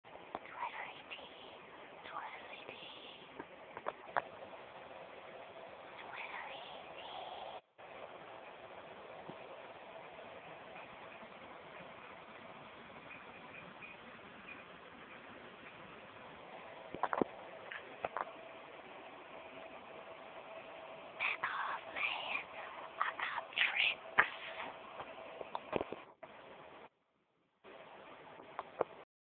Balloons attack...
In an eerie new development, balloons from another planet attempt to take over an air conditioner at a hotel.